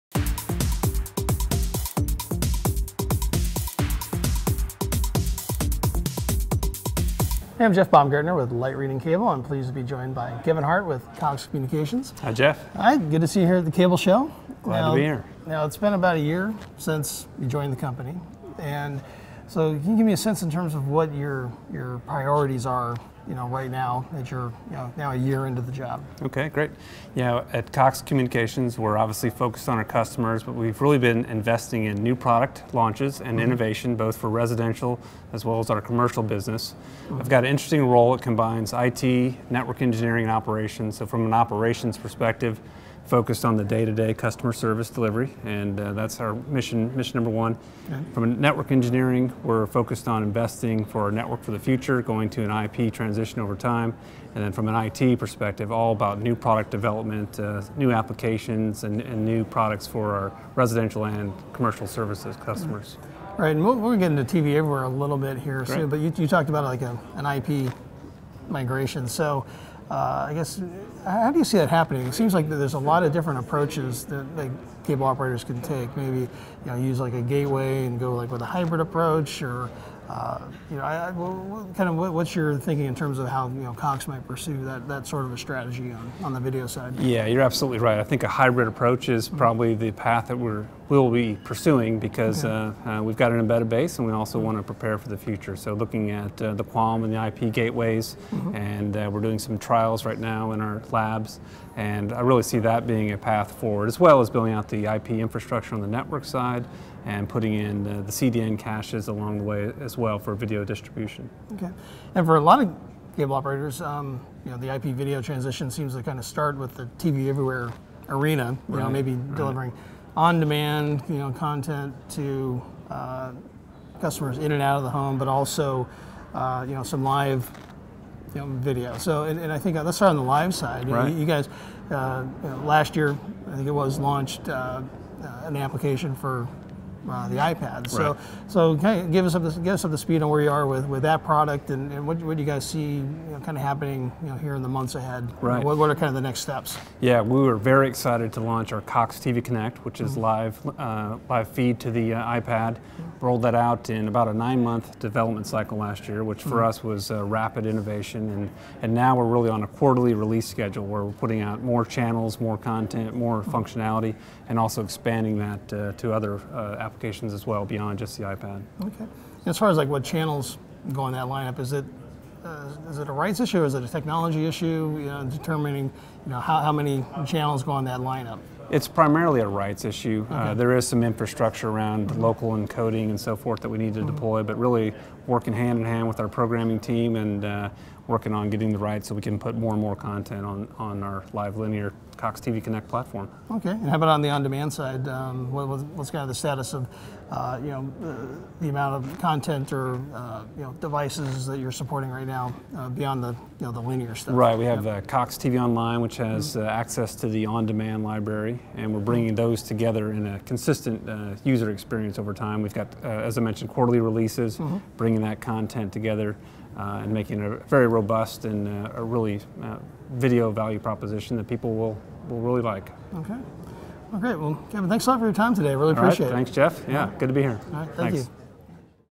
Setting Cox's Priorities
Cox Communications EVP & CTO Kevin Hart explains what's at the top of his to-do list and offers an update on the company's TV Everywhere and IP video strategies